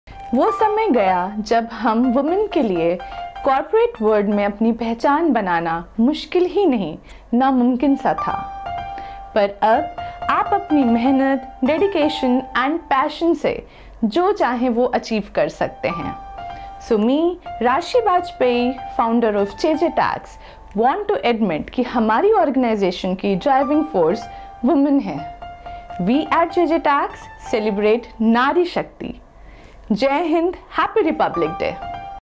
Happy Republic day 2023 - Word by CEO
On the occasion of India's 74th Republic Day, the PM articulated a vision that in the upcoming years "Nari Shakti" would play a vital role in the country's development. We at JJ Tax believe that women have always been, are currently, and will always be an integral part of developing this nation - and we have witnessed this in our own company. 80% of our employees are female, and they have been a driving force in our organization as quoted by our CEO.

This Republic Day, we want to give a big shoutout to all the women who have been contributing to society - whether they be homemakers, entrepreneurs, pilots, or anything else. Thank you for everything that you do! ✨❤️